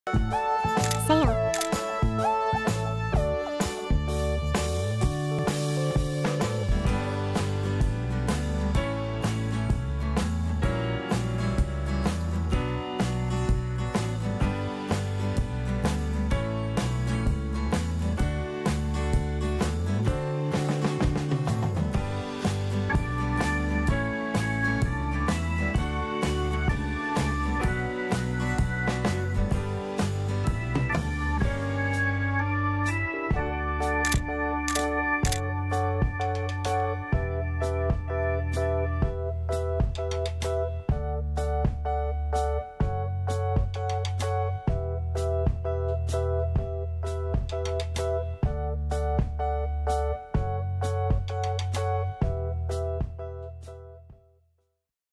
Super NES Classic Edition + Zelda Mystery Collector Box | jSALEj #17
The Super Nintendo Entertainment System: SNES Mini Classic Edition and Limited Edition The Legend of Zelda Culturefly Collector Mystery Box.

JSALEJ Company
store.JSALEJ.ca